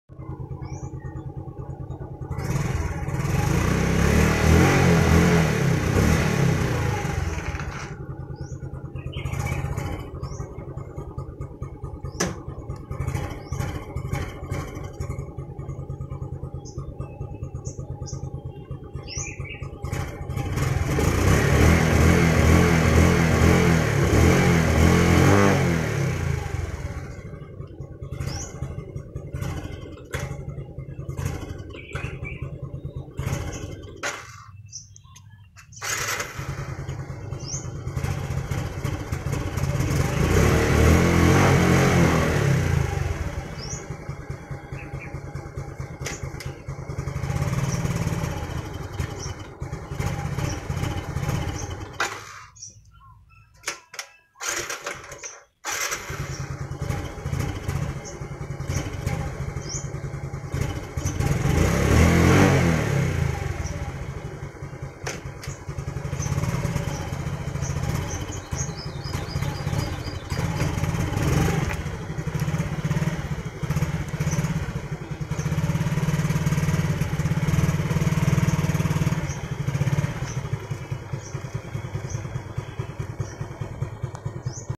Suzuki Smash not starting fast
Patulong po. Ano kaya problem nito. Matagal po patakbuhin yung Smash ko. Kelangan pa painitin ng mga 10 to 15 mins before magamit talaga. Kasi pag bitin namamatay ng kusa yung makina. Hirap ako pag naka park ng mahabang oras kasi matagal xa patakbuhin nakakahiya tuloy sa mga kasabay.

PS: Kakalinis lang ng Carb at Air Filter. Bago din yung spark plug.

Ano kaya problem nito?